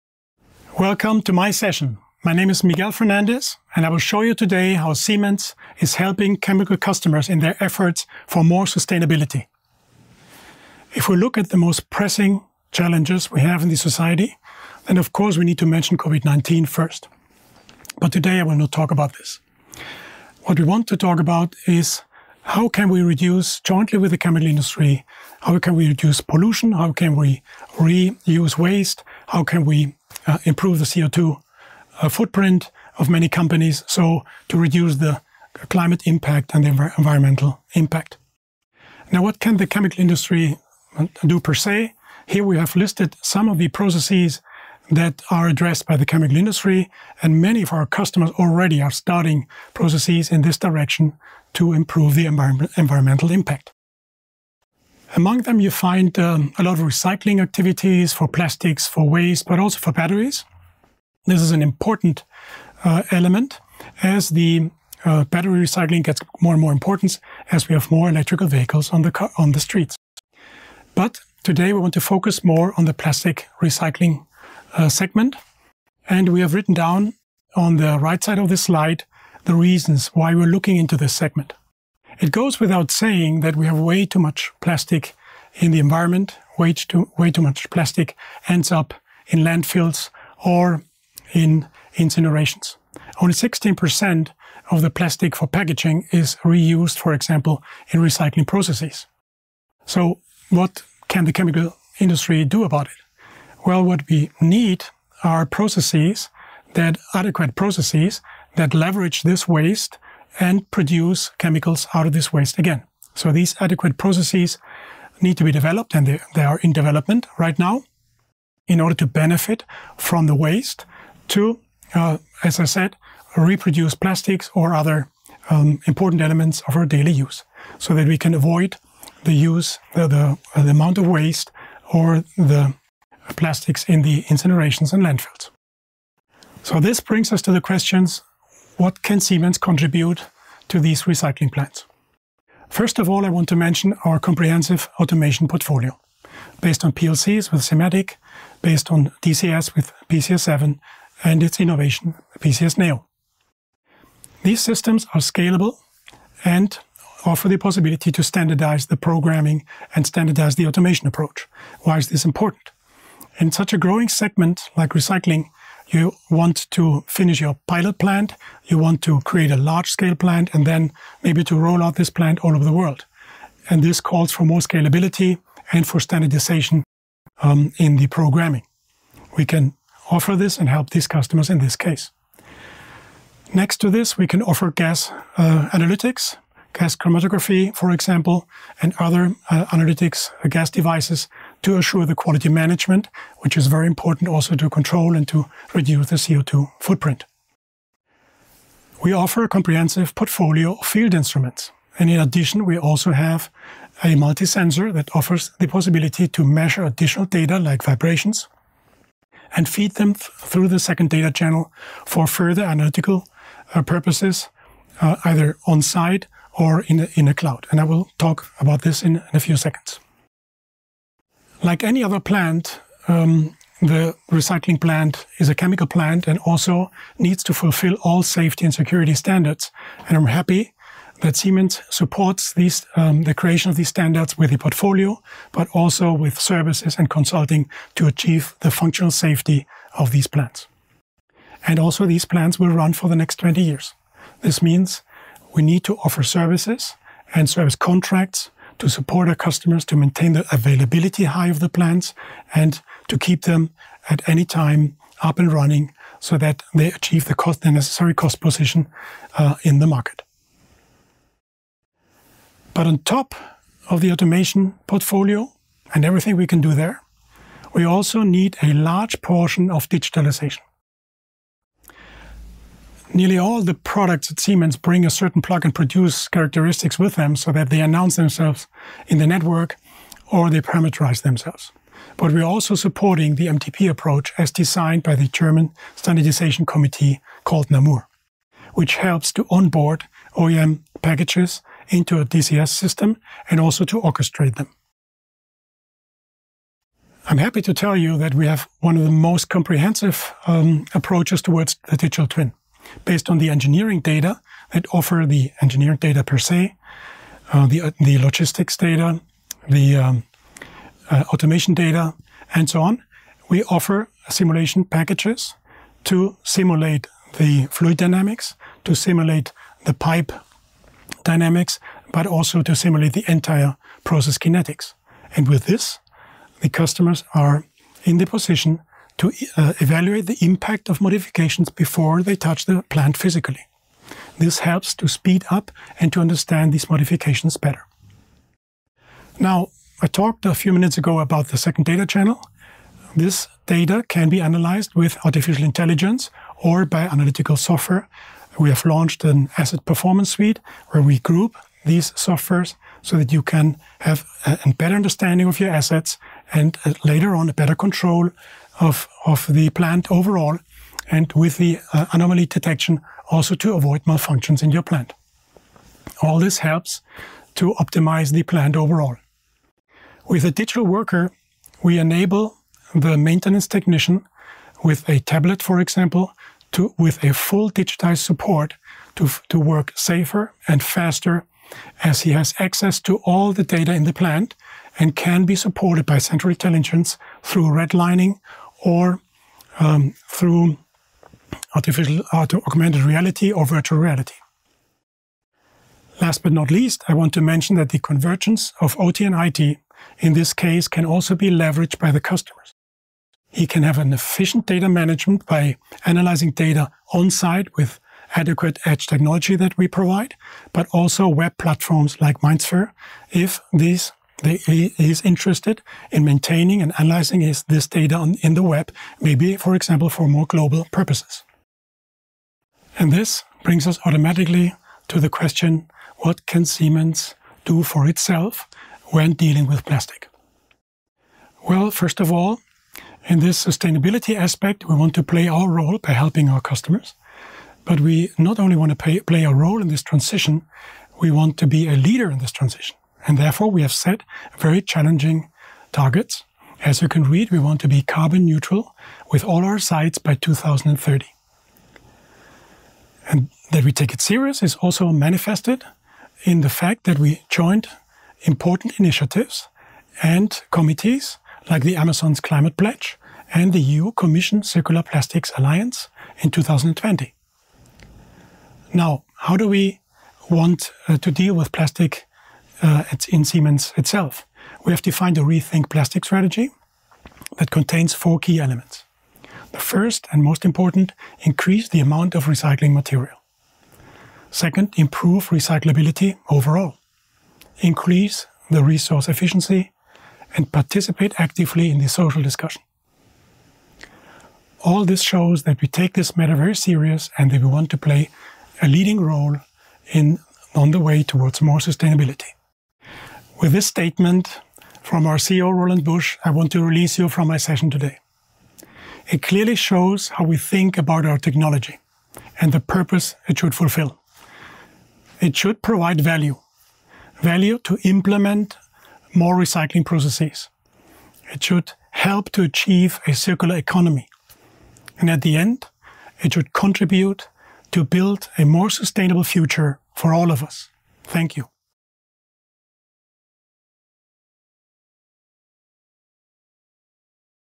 Chemistry is going cyclic – Closing the loop for a sustainable future
Siemens focus on increasing sustainability in the chemical industry. We attend and assist our customers to create innovative and environmentally friendly applications. We work together to develop and implement solutions that keep resources in the loop as long as possible, thus simultaneously minimize the need for dumping them. This ends up in beneficial solutions to optimize our resource needs by reducing, reusing and recycling materials and energy.
Do you want to benefit from over 170 years of experience? Get in touch with us! At:

Siemens.